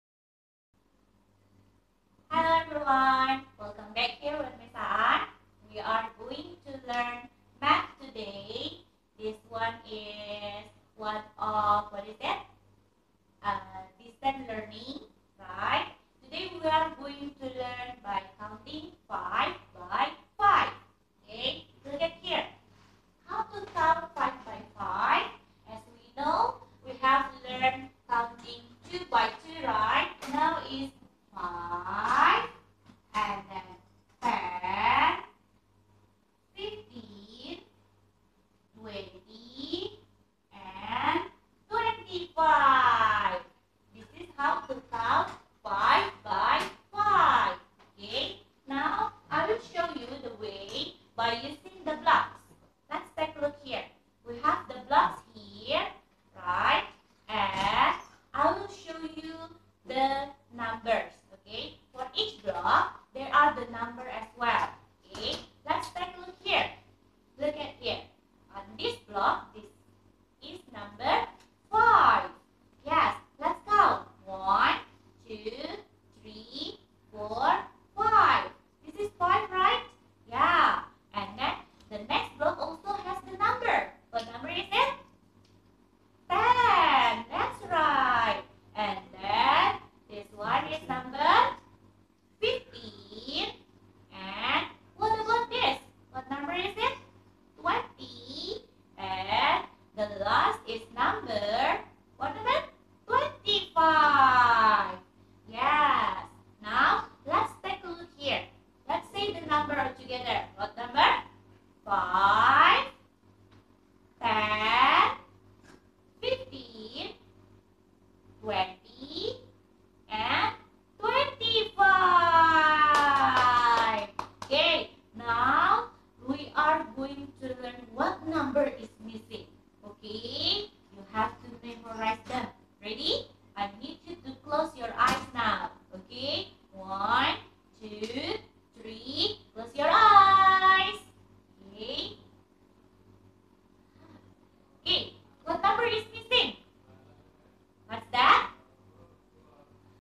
Math K2 Count by 5 part 1
Distance learning video for Starfish School Kindergarten 2. This video is made due to the Covid 19 so students can learn from home. Kindly teach your child at home for now.
Hope to see all of you soon.
Ms. Aan